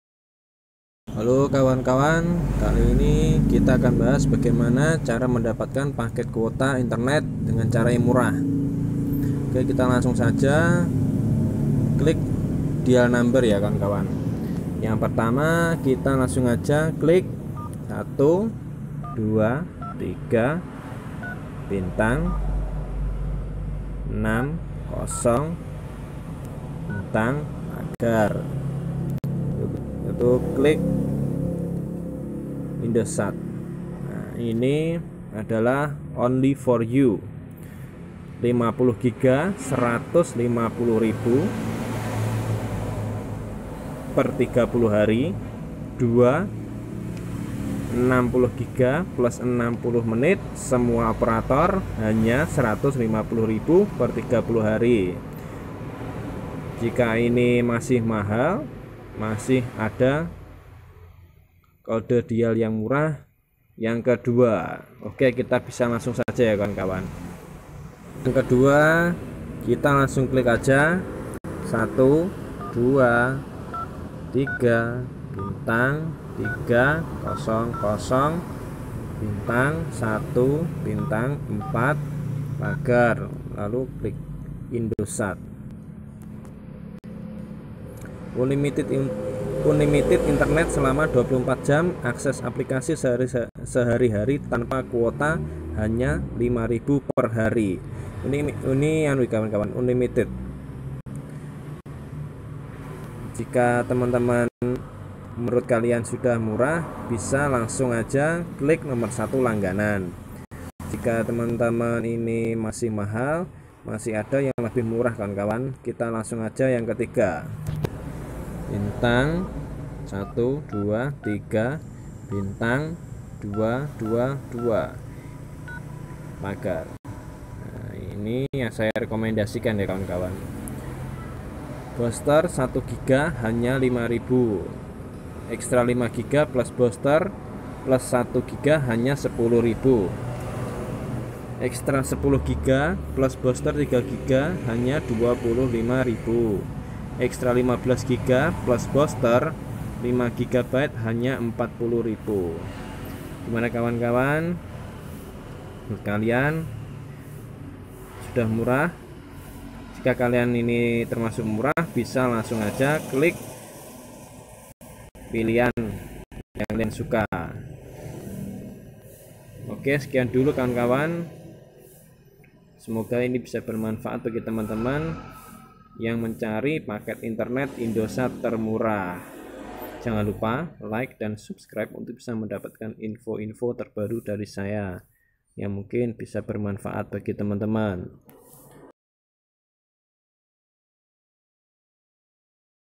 Paket murah indosat # terbukti
Okey kawan kawan ingin tau kode dial indosat termurah dan terbaru simak vidio ini ya..